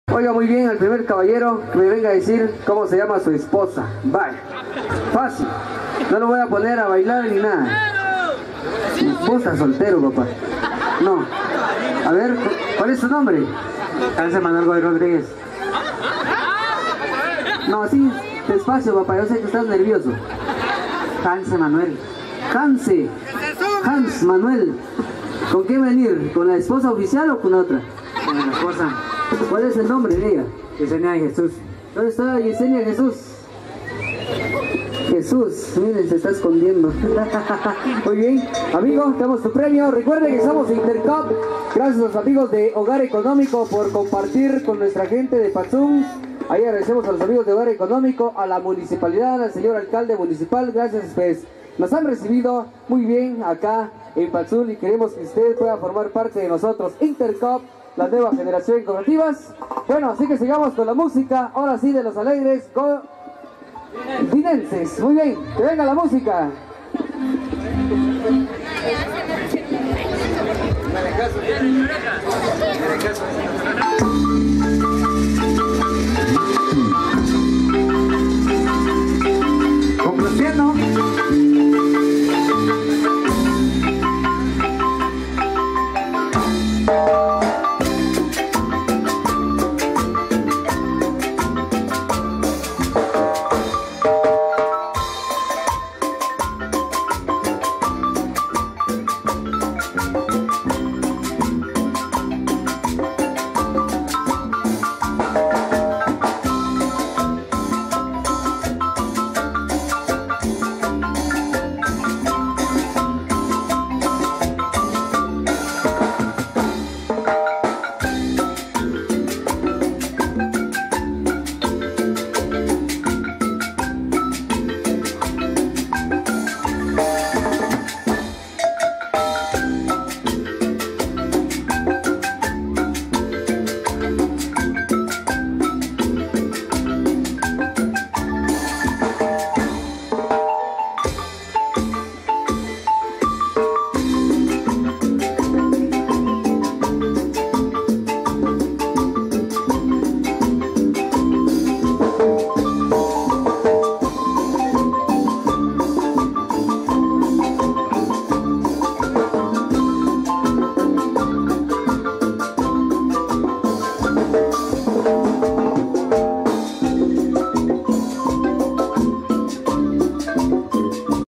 Alegres Godinences en la feria titular de Patzún
Patzún, la música cortesía de "Hogar Económico" durante la fiesta titular en este municipio estuvo a cargo de "Alegres Godinences"